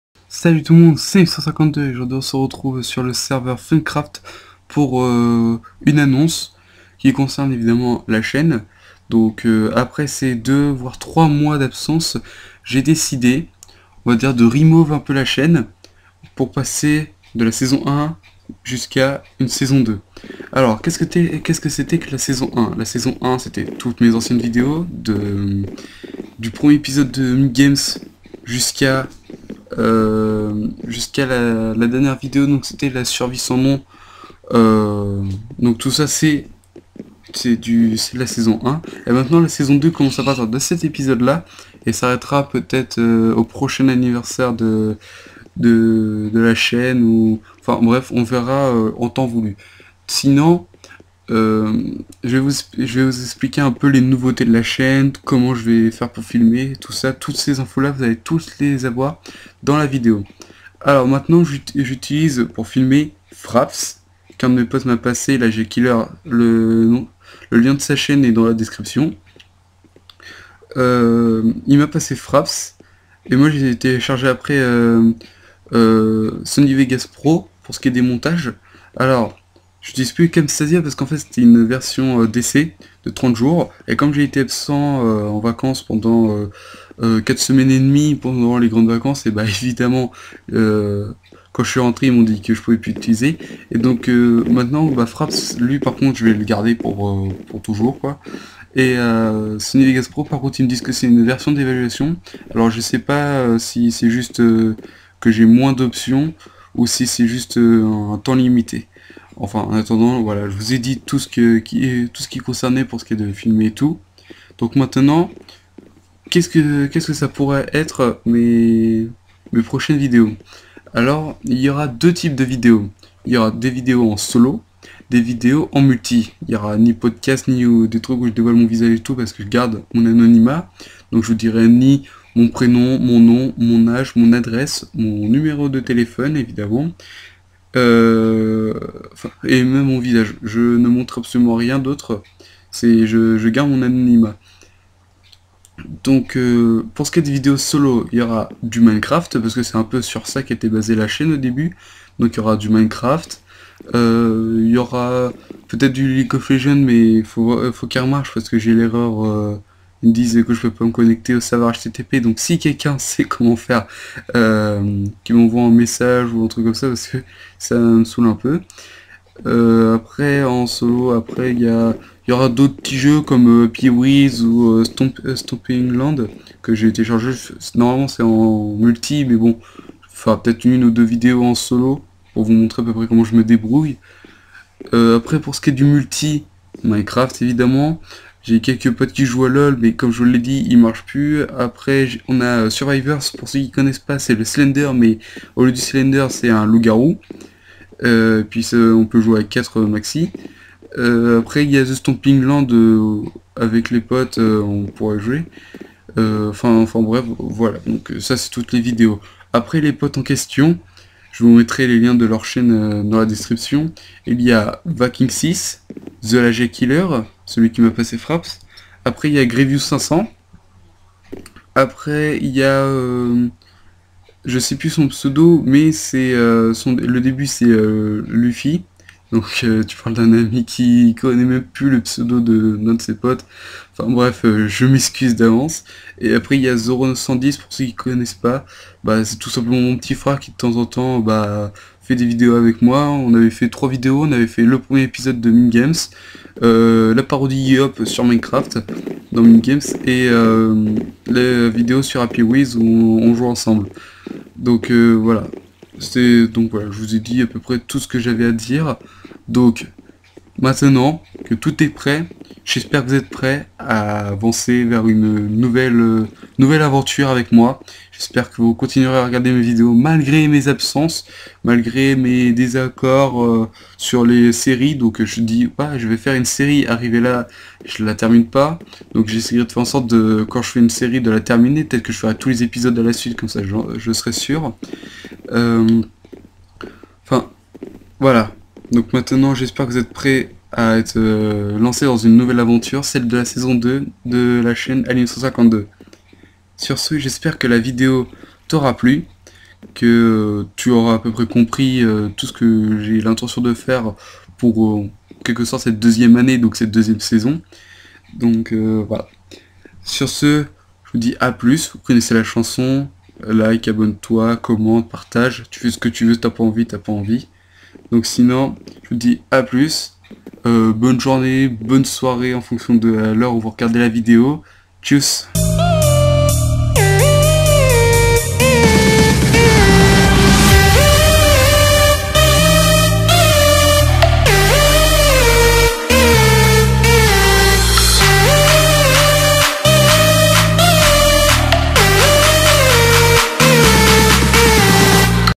ANNONCE SPECIALE : SAISON 2 DE LA CHAINE
Slt tout le monde ! Aujourd'hui, on passe à la saison 2 de ma chaîne ! Pour cette nouvelle saison, du solo et du multi, du Minecraft et du fun, des fous rires et des fails !

Bon visionnage !